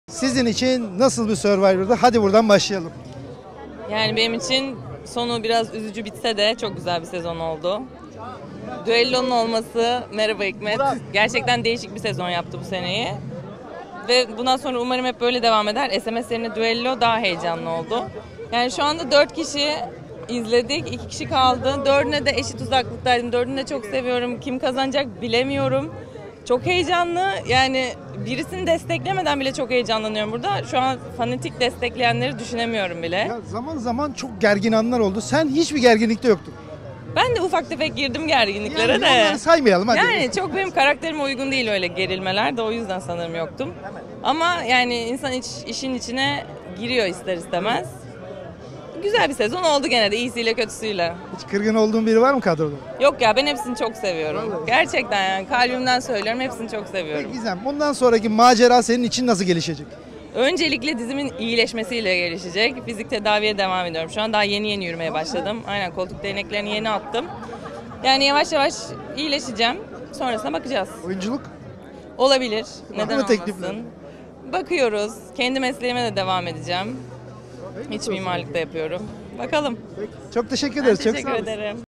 ÖZEL HABER: Düello yarışı çok etkiledi
Survivor yarışmacılarından Gizem Memiç, Magazin Burada mikrofonlarına dikkat çeken açıklamalar yaptı.